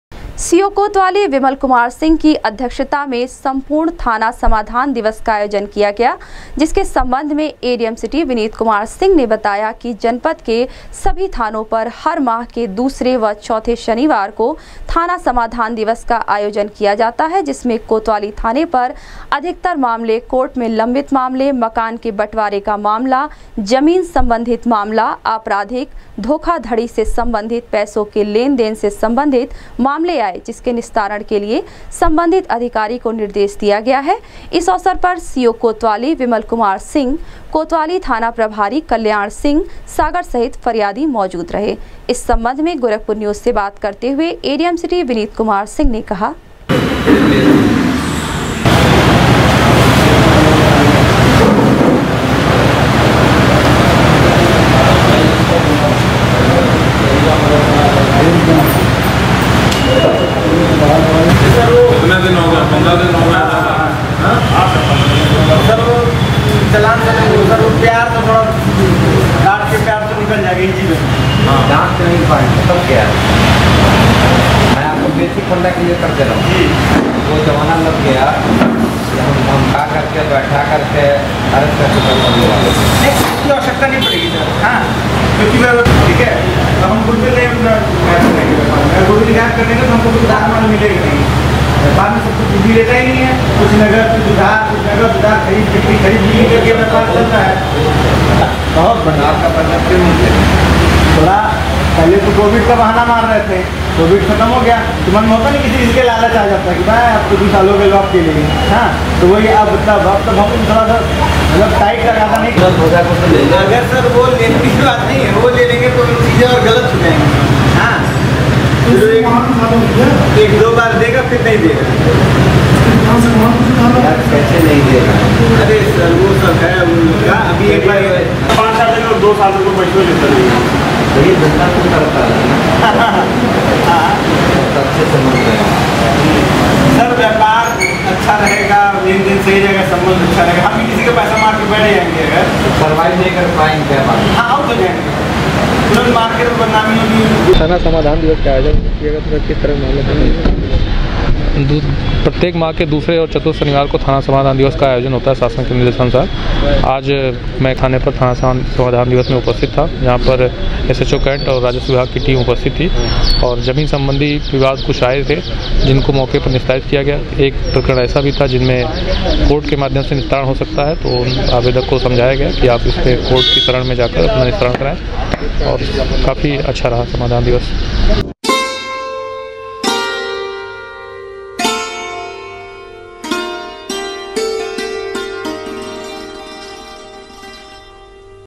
कोतवाली थाने पर सीओ विमल कुमार सिंह की अध्यक्षता में संपूर्ण थाना समाधान दिवस का किया गया आयोजन
कोतवाली थाने पर सीओ विमल कुमार सिंह की अध्यक्षता में संपूर्ण थाना समाधान दिवस का किया गया आयोजन,कहा,अधिकतर मामले मकान के बॅटवारे, पैसों के लेनदेन से आये सम्बंधित,मामलों के निस्तारण के लिए अधिकारियों को दिए निर्देश